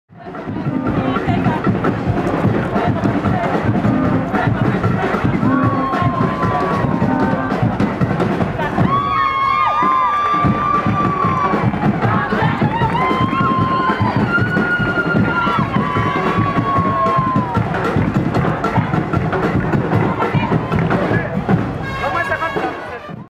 LYANNAJ' Parade du Dimanche Gras le 19 Février 2012 à Pointe-Pitre
LYANNAJ' groupe qui participe à la parade du Dimanche Gras en GUADELOUPE le 19 Février 2012.
CARNAVAL aux ANTILLES.